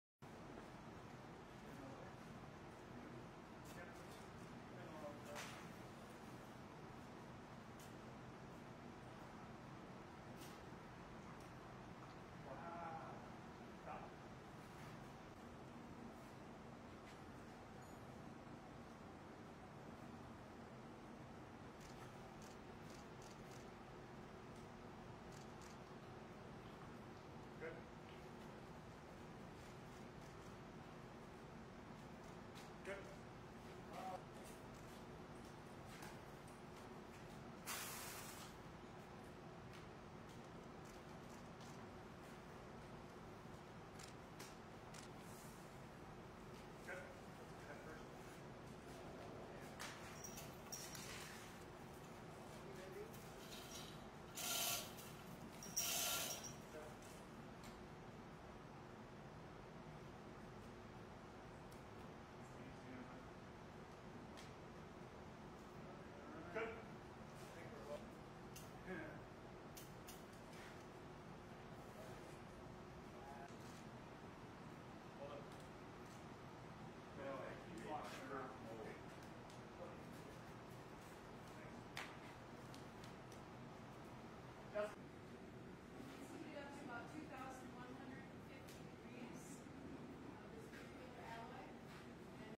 2020-01-14 Be sure to join us on Monday for the Martin Luther King, Jr Statue Dedication!
Bronze casting of Porter Williams dream of Martin Luther King Jr shaking hands in Sioux Falls, SD.